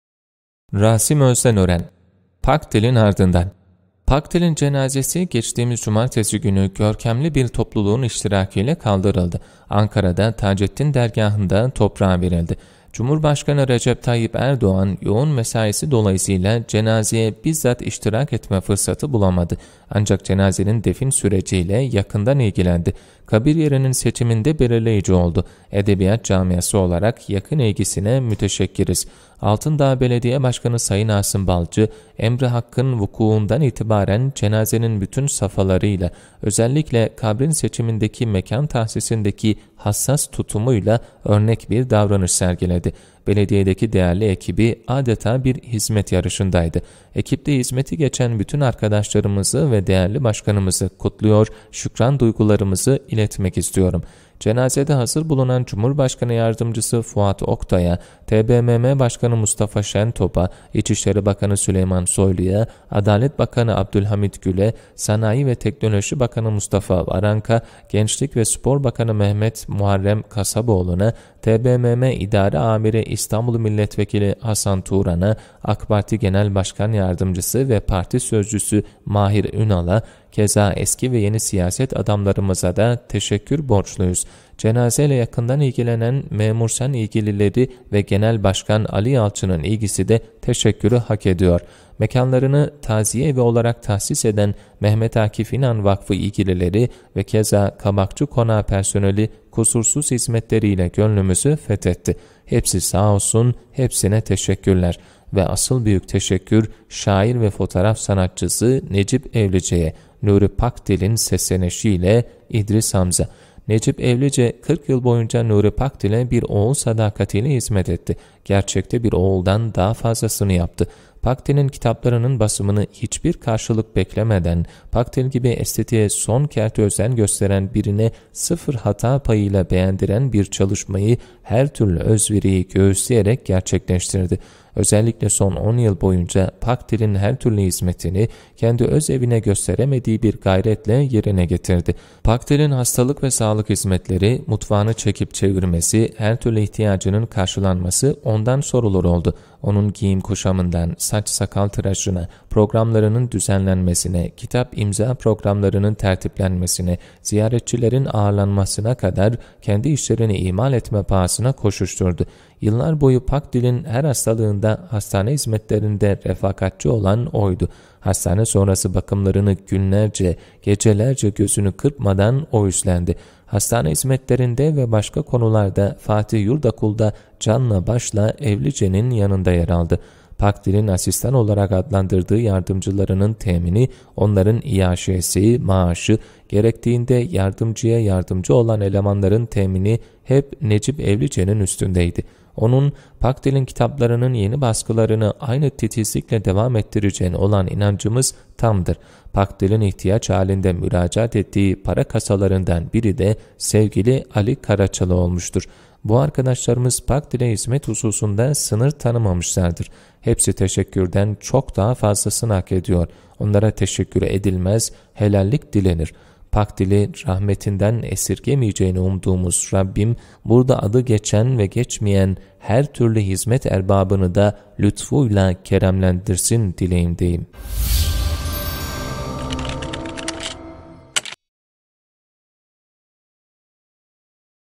Rasim Özdenören - Pakdil’in ardından - 24.10.2019
Pakdil’in cenazesi geçtiğimiz cumartesi günü görkemli bir topluluğun iştirakiyle kaldırıldı. Ankara’da Tacettin Dergâhı’nda toprağa verildi.